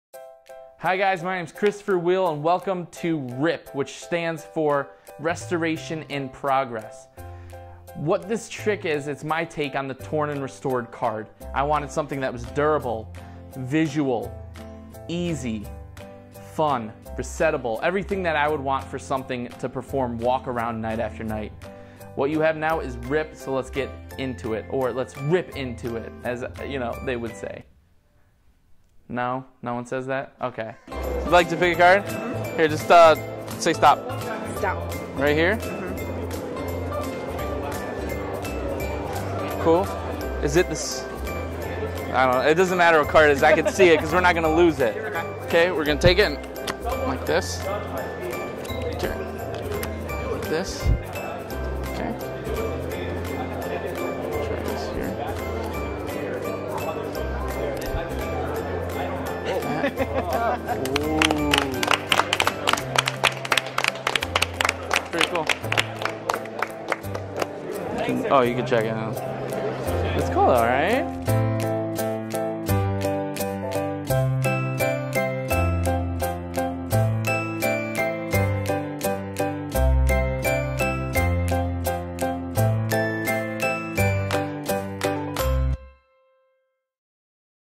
RIP | TRAILER
There are many torn and restored card effects on the market. Most are difficult, multi-phased, and angle-sensitive. Pros and cons for each one.

The advantage of RIP is that the ENTIRE effect happens in ten seconds. You casually tear a card to pieces, then shake the pieces. They VISIBLY restore, front and back.

What you receive with RIP is a GORGEOUS handmade gaff that has two positions: in the open position it looks exactly like a messy pile of pieces, front and back. When it flips (and locks) closed, it is a normal card, front and back. There are no threads or parts to break: it's practical and completely reliable.

Custom hand-made gimmick, ready to perform straight from the box
Includes 1 hour, 20 minute DVD with many variations
Full instruction on how to make your own, additional gimmicks
Surprisingly easy to perform!
RIP was a sellout at the Blackpool Convention, and we witnessed dozens of people walking around "flipping" the gimmicked card closed to create the illusion of an instant restore. This gimmick is fun. It works itself, is easy to do, and has no bad angles. If you're looking to add a quick, visual Torn and Restored moment into your card set, RIP is just for you.